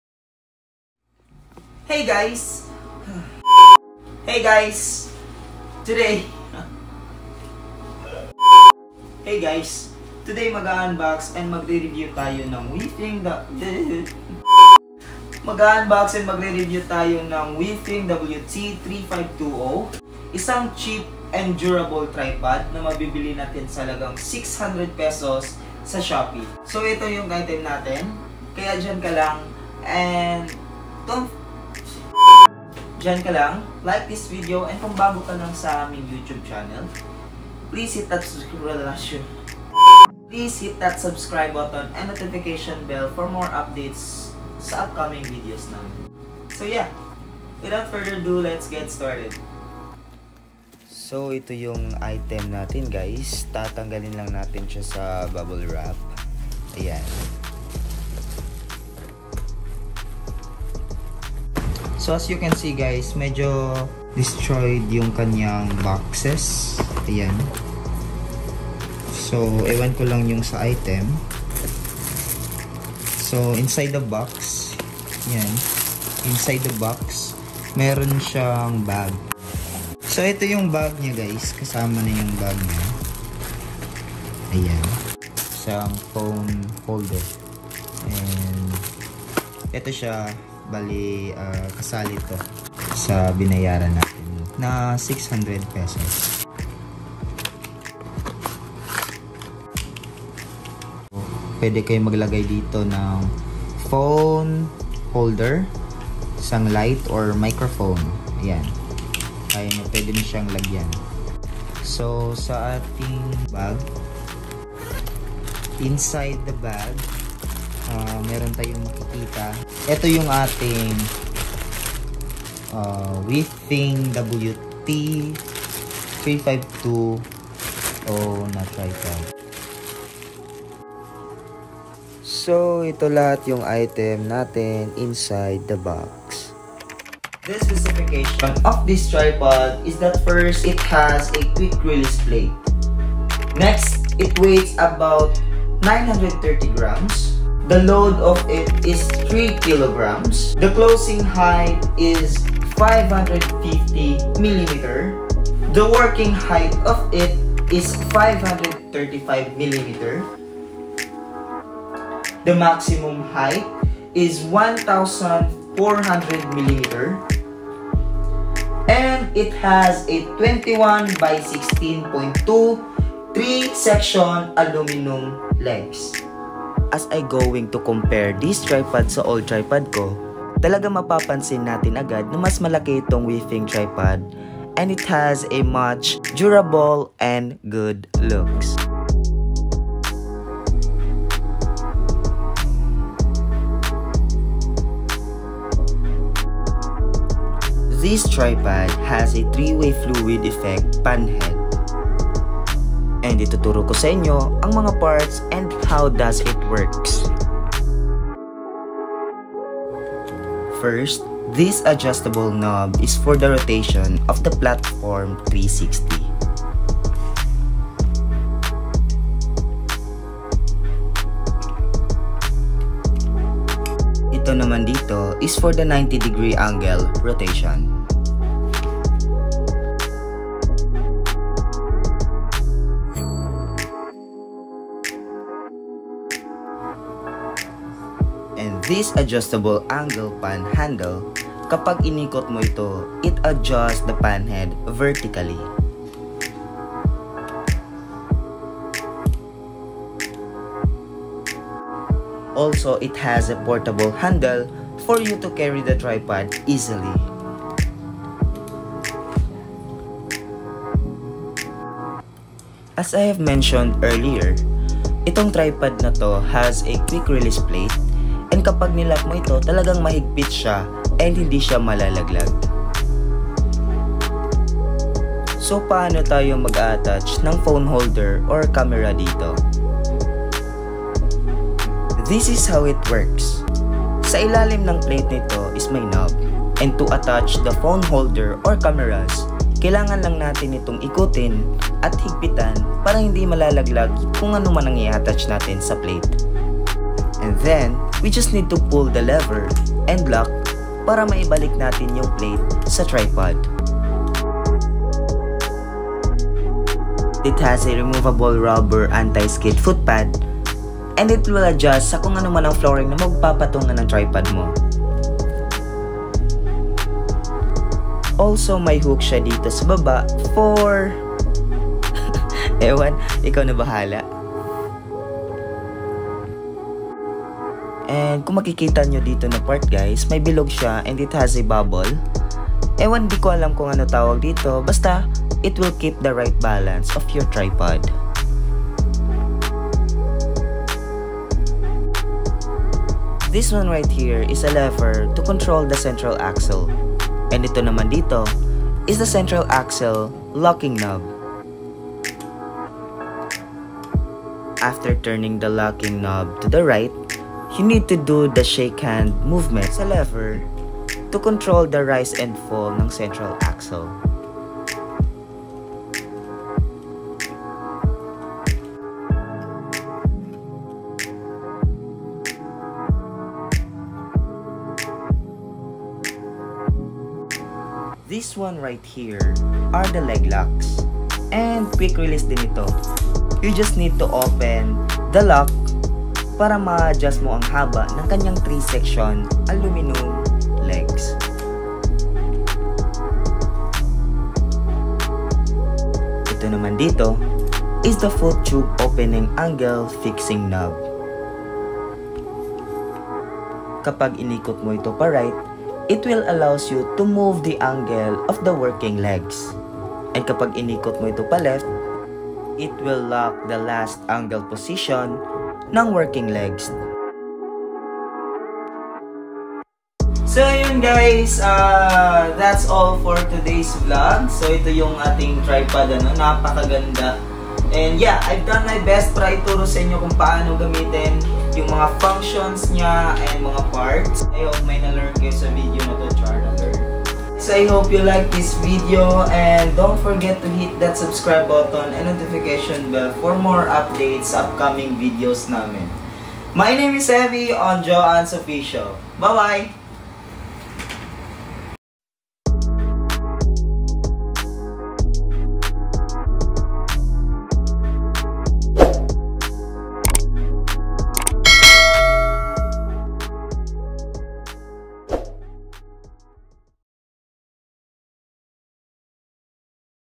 CHEAP AND DURABLE TRIPOD // WIEFENG WT 3520 // UNBOXING AND REVIEW //
YES I’VE BOUGHT NEW TRIPOD FROM SHOPPEE
AND ITO YUNG WIEFENG WT 3520 TRIPOD

SA HALAGANG P600 TALAGANG MAPAPAHANGA KA
SA PARTS AND DURABLE MUCH GOODLOOKS NITO

KAYA KUNG NAG HAHANAP KA NG CHEAP AND DURABLE TRIPOD I RECOMMEND THIS PRODUCT DAHIL NAPAKA GANDA NGA NAMAN NITO!

SO YEAH I HOPE YOU LIKE AND ENJOY THIS VIDEO

SA MGA BAGO PALANG SA YT CHANNEL NAMIN